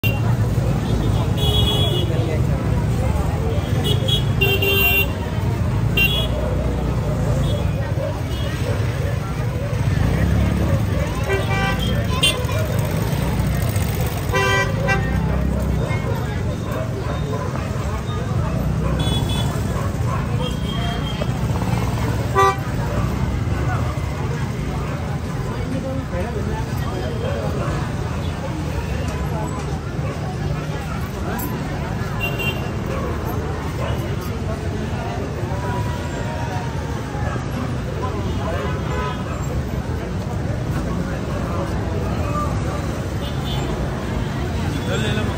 September 28, 2023
The night before the Indra Jatra festival in Kathmandu.